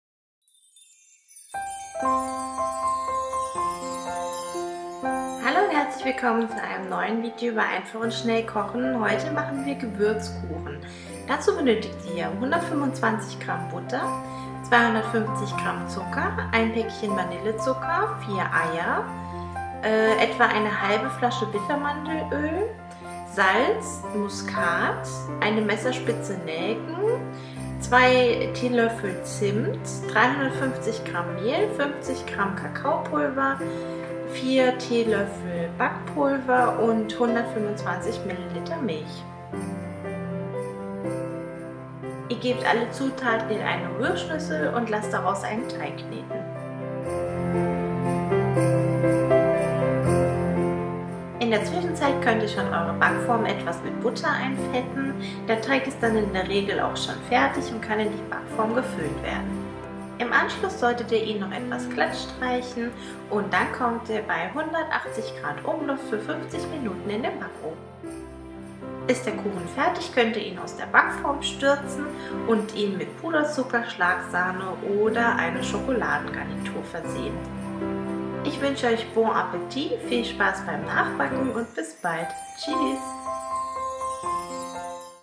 ESK: Weihnachtlicher, schokoladiger Gewürzkuchen
Rezept:
125g Butter
250g Zucker
1 Päckchen Vanillezucker
4 Eier
1/2 Flasche Bittermandelöl
Salz
Muskat
1 Msp. Nelken gemahlen
1 TL Zimt
350g Mehl
50g Kakao
3-4 TL Backpulver
125ml Milch
Für die Dekoration könnt ihr wahlweise Puderzucker, Schlagsahne oder Schokoladengarnitur nehmen.